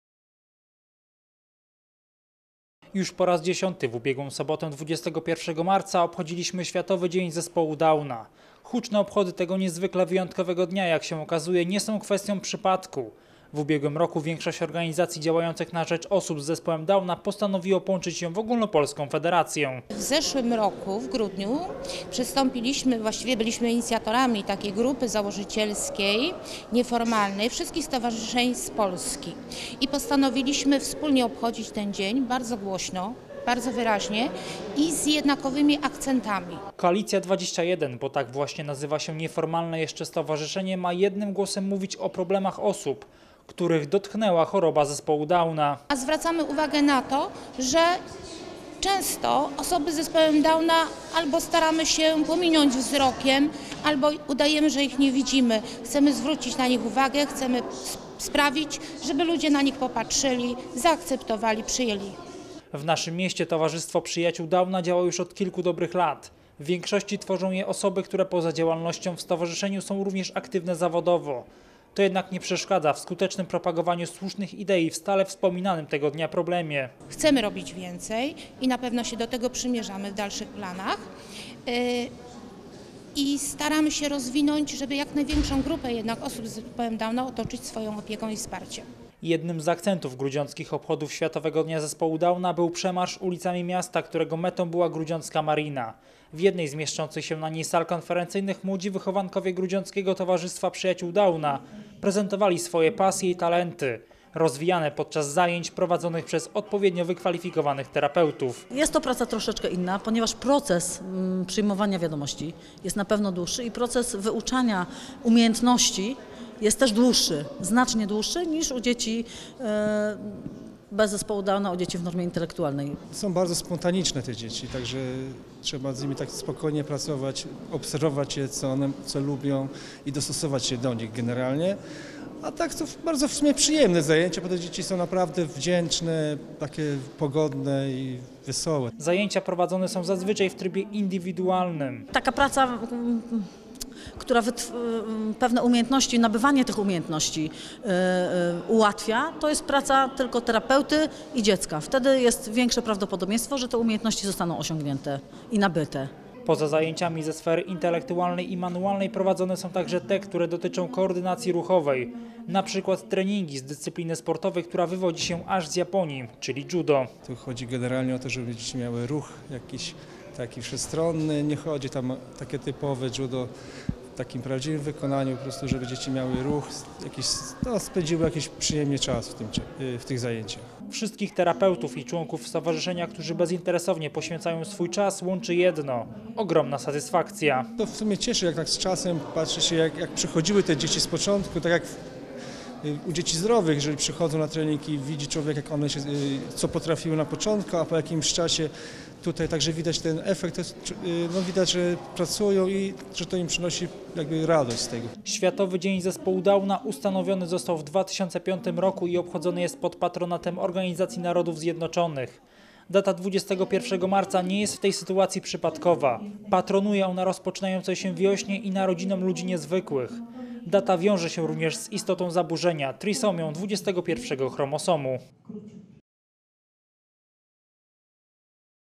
23-03-2015 Huczne obchody X Światowego Dnia Zespołu Downa
Już po raz dziesiąty obchodzony był Światowy Dzień Zespołu Downa. Z tej okazji w naszym mieście odbył się przemarsz przez nasze miasto i specjalna konferencja w Marinie. Organizatorem tego święta w naszym mieście było Towarzystwo Przyjaciół Downa.